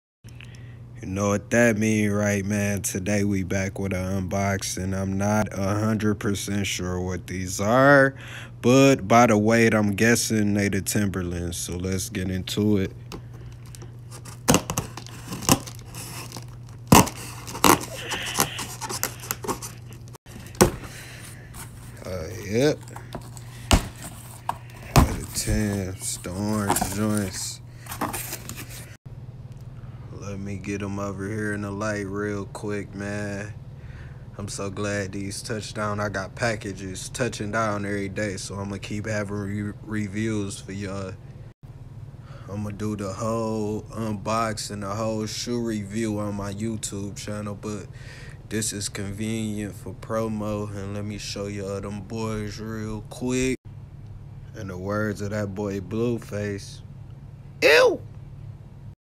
UNBOXING OF THE LIMITED EDITION ORANGE TIMBERLANDS YOU CAN ONLY PURCHASE AT DTLR VILLA!!! 📦🧡
Shoe Instagram: @kickgawdz